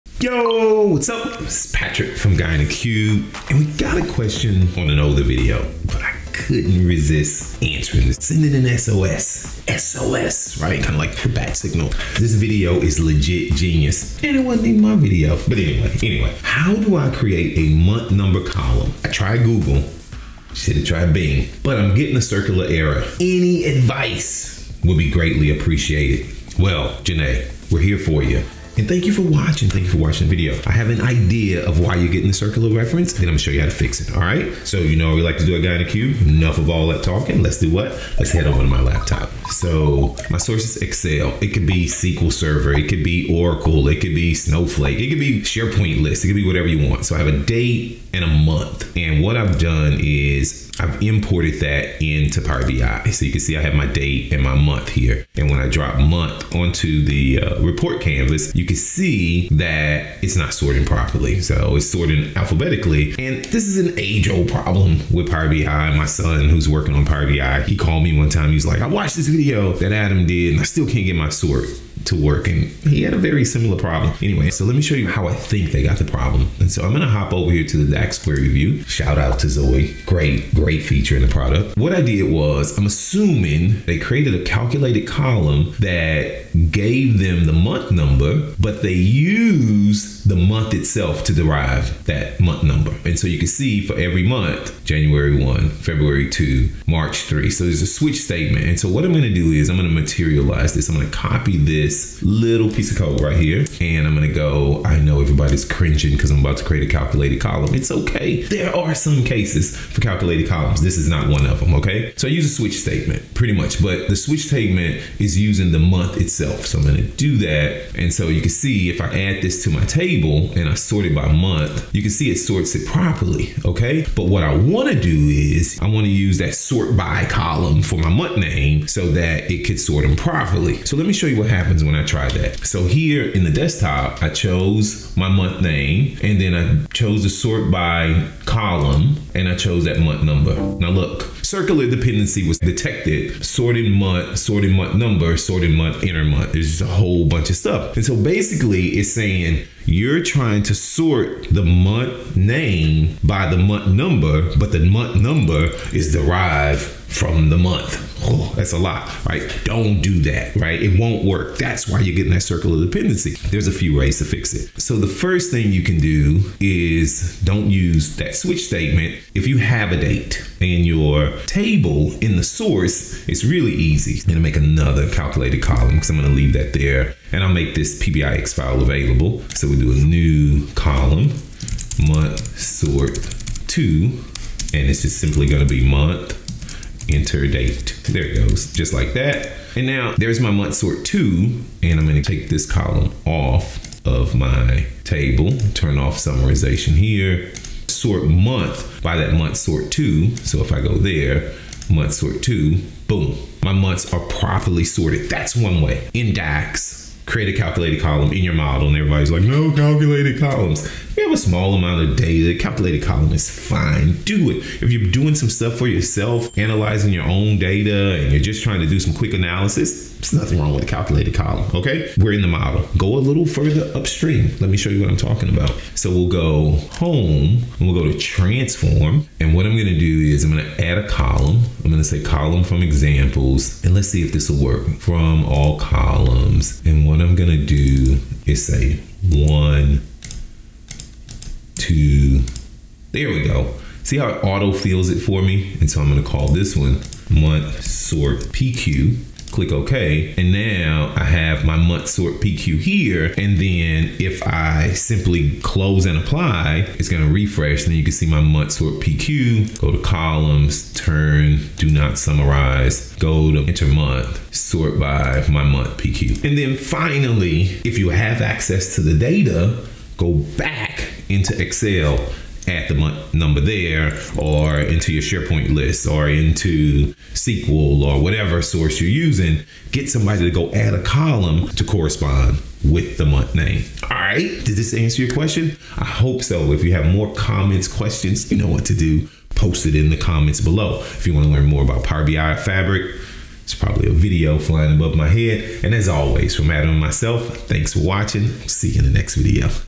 Conquer Circular Dependencies: Sort Months Effortlessly in Power BI
Are month-sorting errors driving you mad? In this video, Patrick lifts the curtain on why your semantic model keeps throwing that frustrating circular dependency error. Watch step by step as he reveals the fix—plus a game-changing tip on heading off these issues upstream in Power Query or even at the data source. It’s time to banish those Power BI sorting nightmares and reclaim your data sanity once and for all!